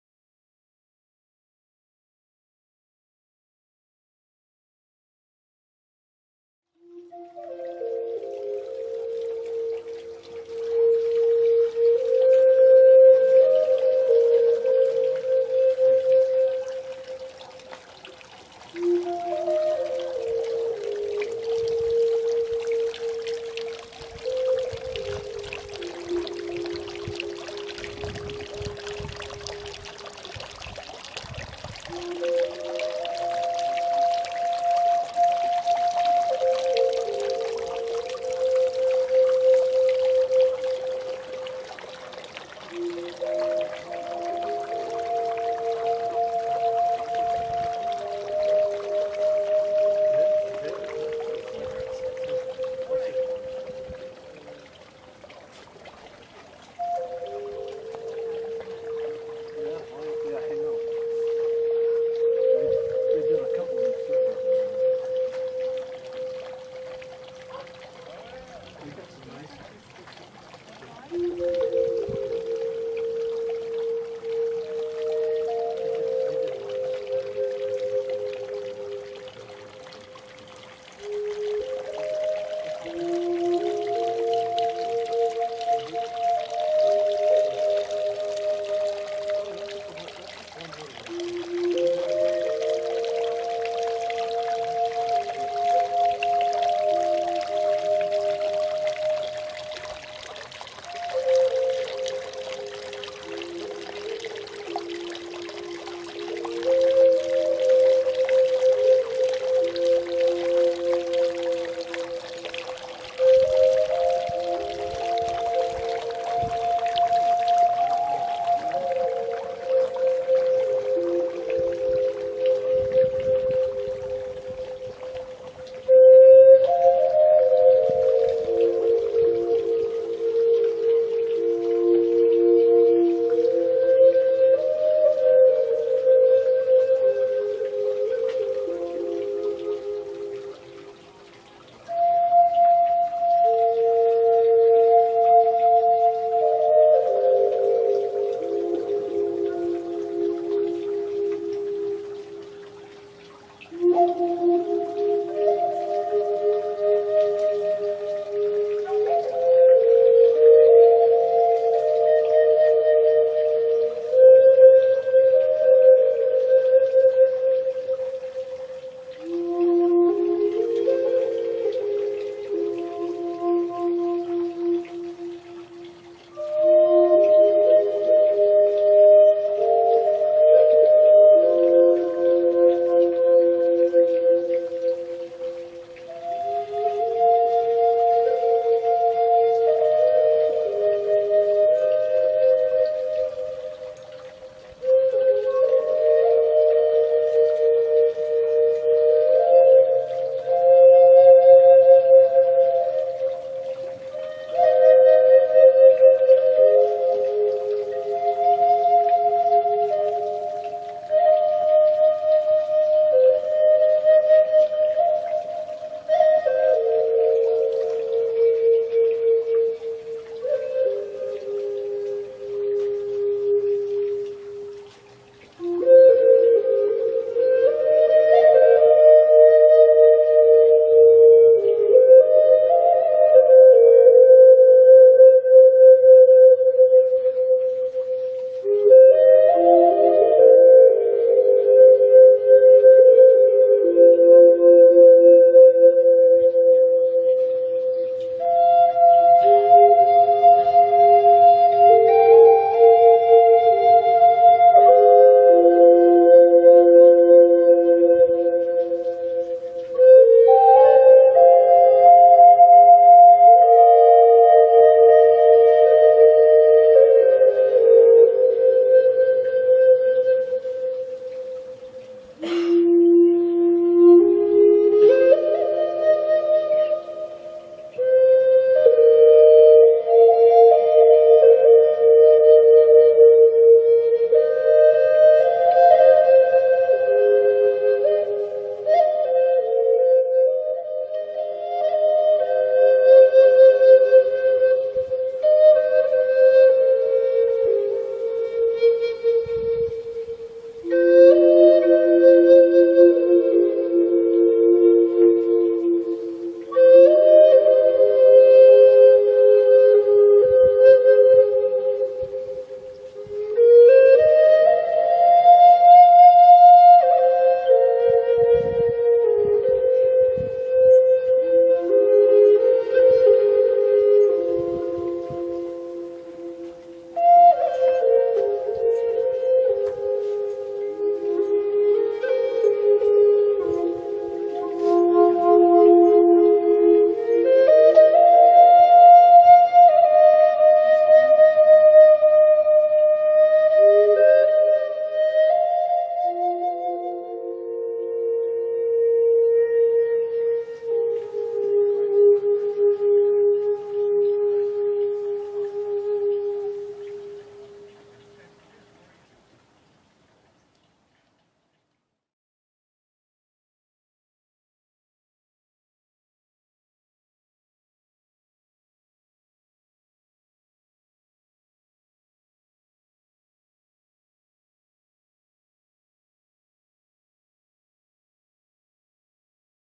Dryad at the Sage and Songbirds Festival
Dryad (Sabine Kurz and Jon Sherman) improvising from the heart on their branch flutes in Penny and Darren Wiemer's garden during the Sage and Songbirds Festival on May 3rd, 2015.   Video filmed by Lani Calvert and used with her kind permission.  For more info about Dryad: